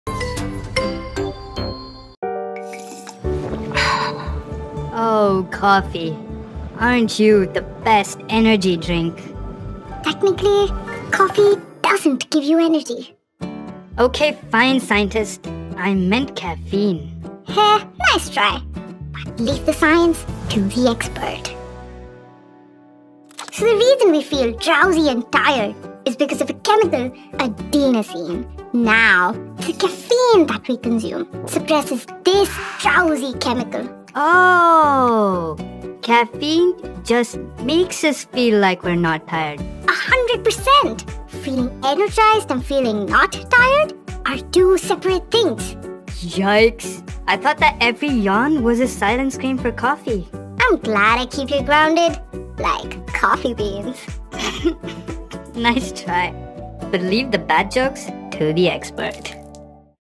The Battle of Caffeine | Why Caffeine make us feel like we are not tired? | BYJU’S Sticky Science
Haven't you ever wondered about how we are always told that the universe has millions and billions of stars, but for some reason, our night sky isn't completely filled with stars? Shouldn't it be? Well, turns out a lot of forces of the universe are in play to keep these sparkling hidden from us. For example, the intensity of light!

Watch this week's Sticky Science as Ruby finds out from Julie why our eyes are only able to perceive the presence of the stars from just our galaxy, the Milky Way!